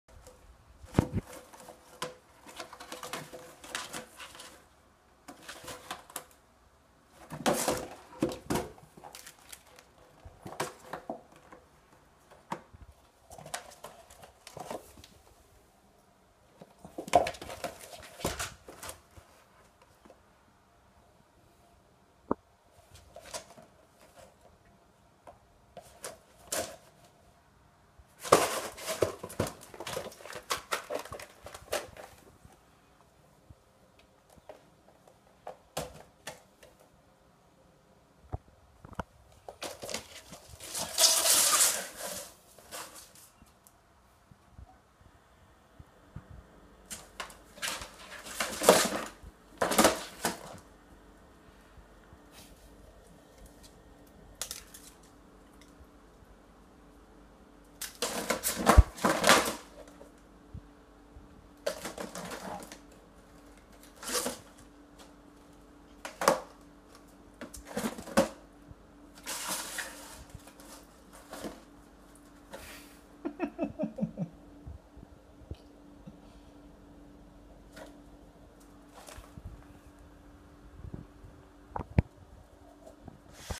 playful Kitten vs cereal box, floor mat, and aluminum foil ball
Kitten vs cereal box & floor mat